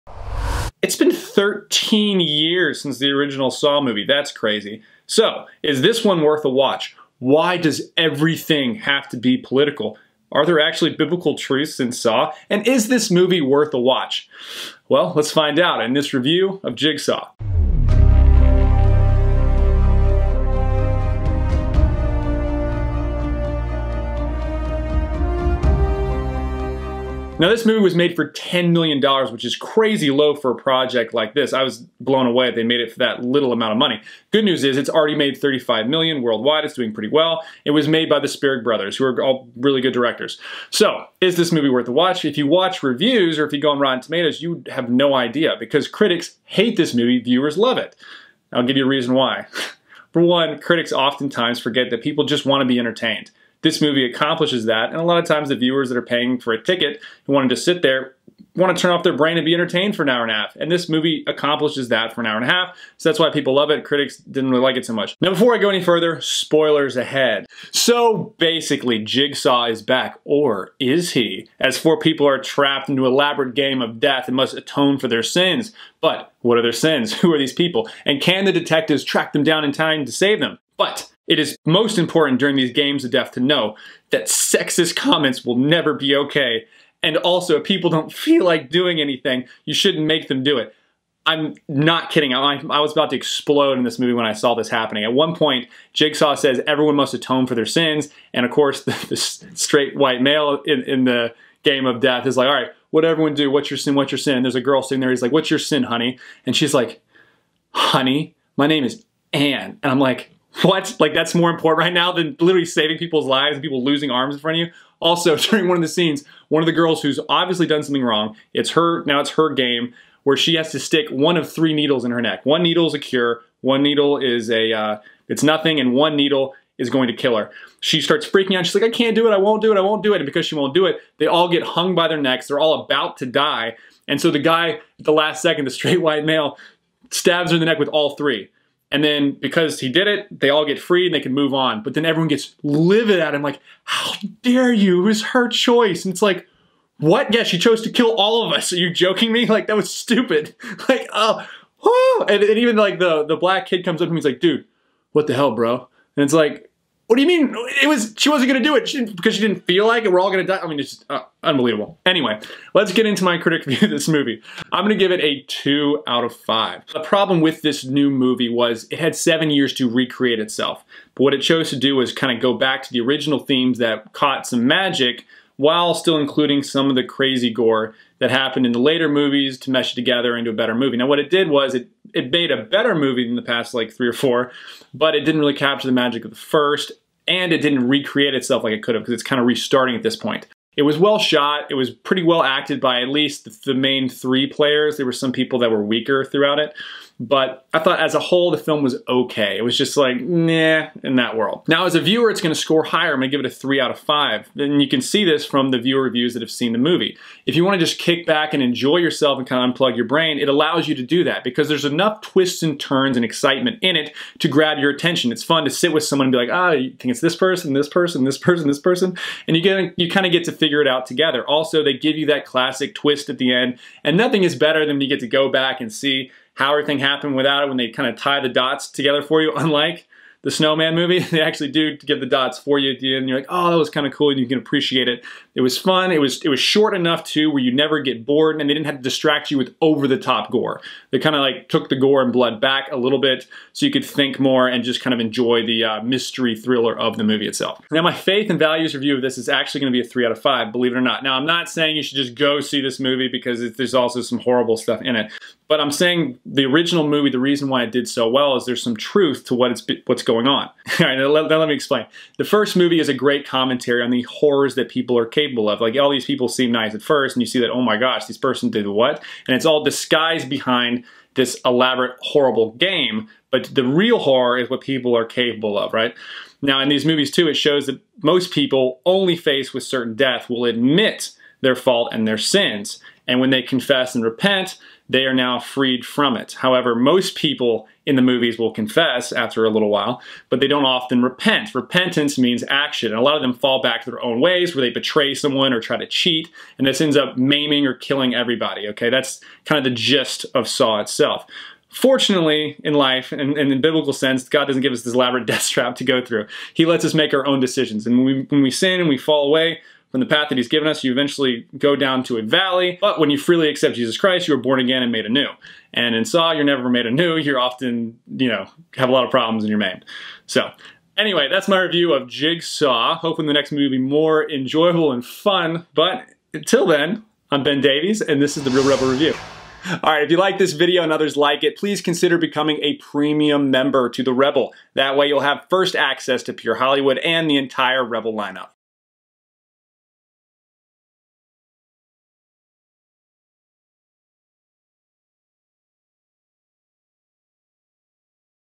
Ben Davies: Why does even "Jigsaw" have to be "political"? | Rebel Reel Reviews
Ben Davies of TheRebel.media: "It's been 13 years since we were shocked (and somewhat impressed) by the original 'Saw.' How does this new chapter stack up, why does everything have to be political, and what does 'Saw' say about redemption?"
PS: Get EARLY access to Ben's videos, PLUS shows hosted by Tommy Robinson, Ezra Levant and MORE!